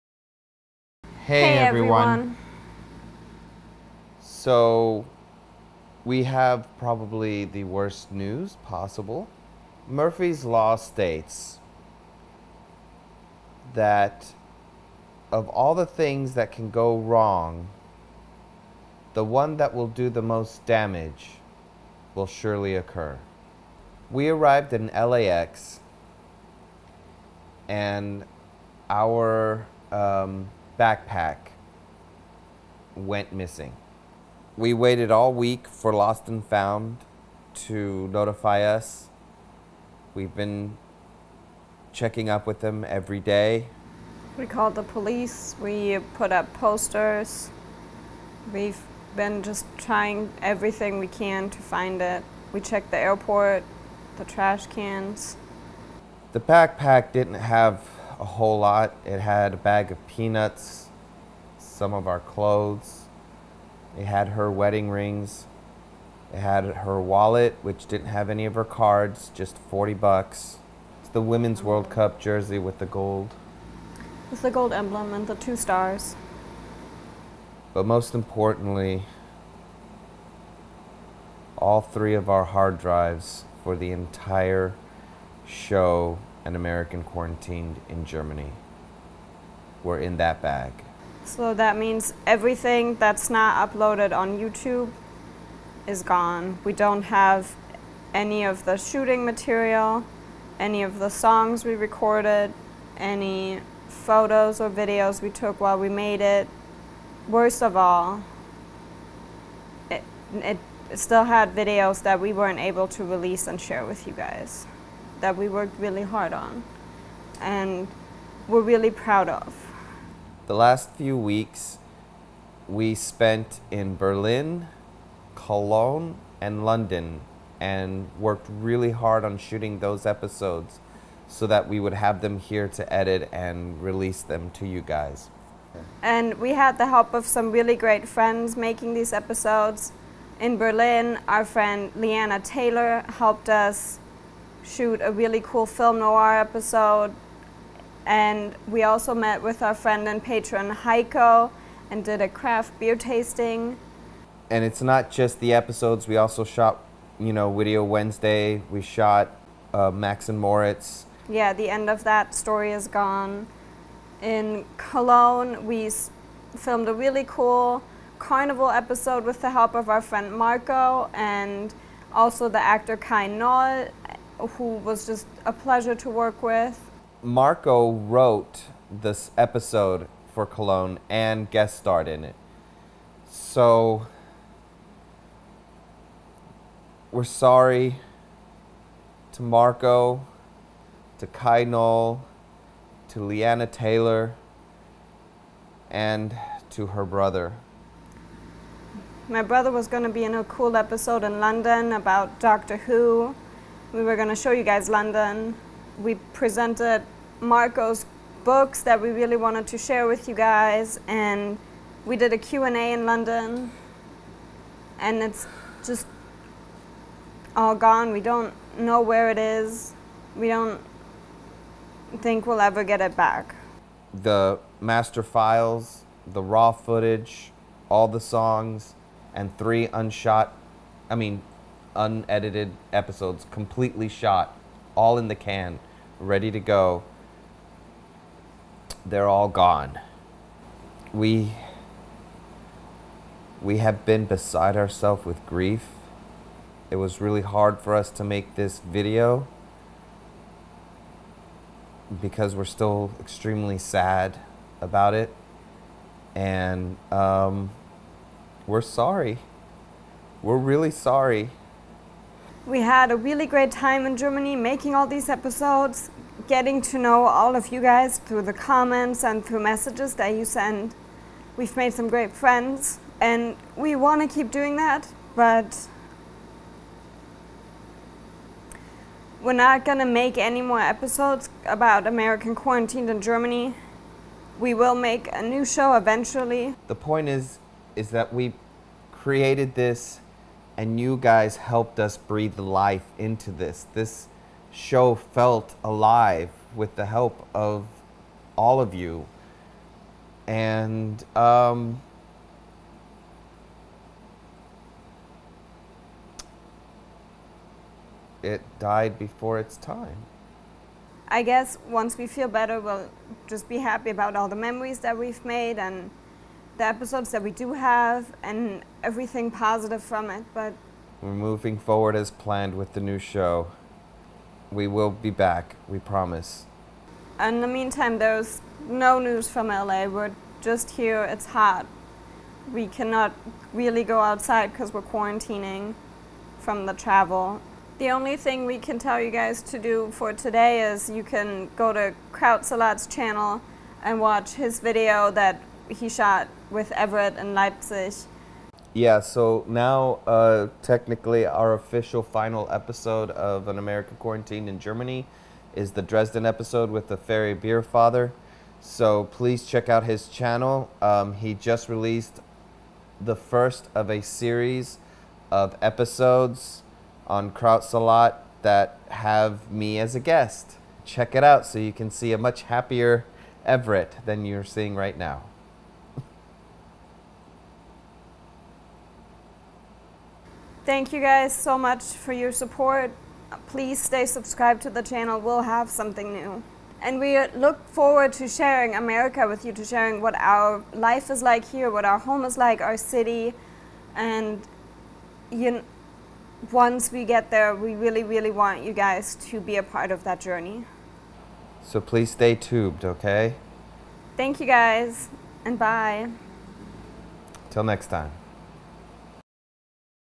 American Quarantined in Germany - Final Quarantine Update
Sad Show Sunday!
We're sorry to see our show come to an end this way and not being able to share our last episodes with you. We worked really hard and we had the great support of awesome friends but unfortunately the universe got in the way.
We hope everyone is save out there, the news are again scary from around the world. Please let us know that you're save and healthy! We hope to see you again soon with better news!